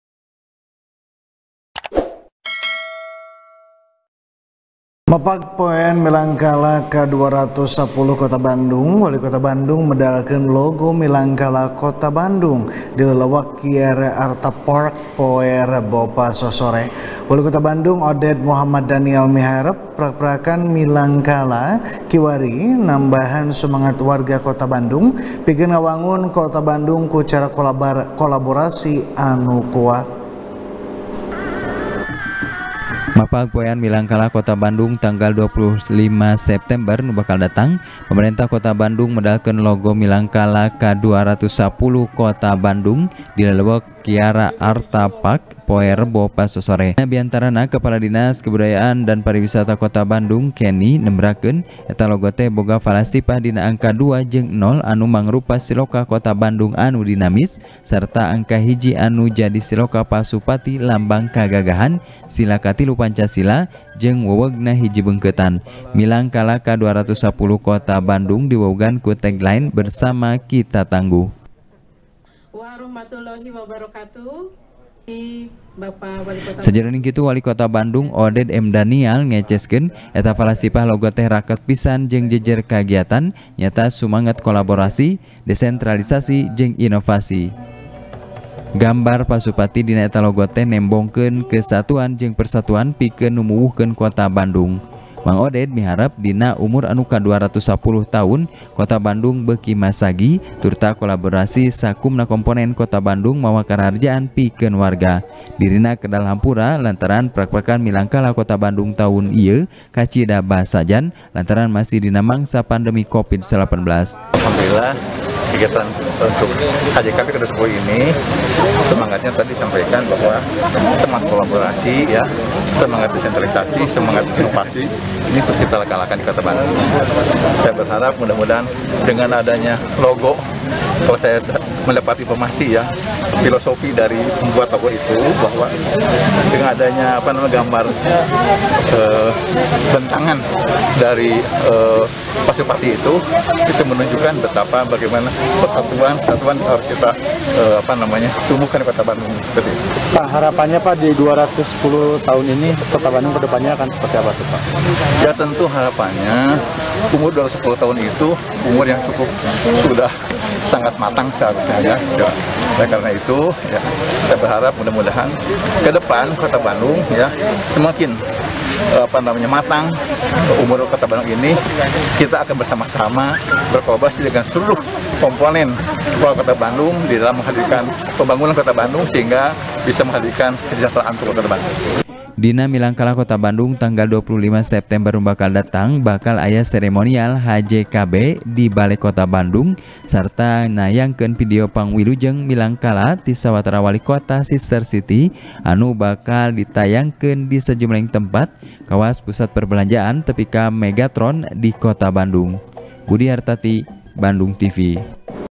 Harepan Mang Oded Dina Milangkala Bandung | TP | BANDUNGTV
TP | BANDUNGTV | DERI FIRMAN | Mapag Poean Milangkala Ka Dua Ratus Sapuluh Kota Bandung, Walikota Bandung Medalkeun Logo Milangkala Kota Bandung, Di Lelewek Kiara Artha Park, Poe Rebo Pasosore. Walikota Bandung, Oded Mohammad Danial Miharep, Prakprakan Milangkala Kiwari Nambahan Sumanget Warga Kota Bandung Pikeun Ngawangun Kota Bandung Ku Cara Kolaborasi Anu Kuat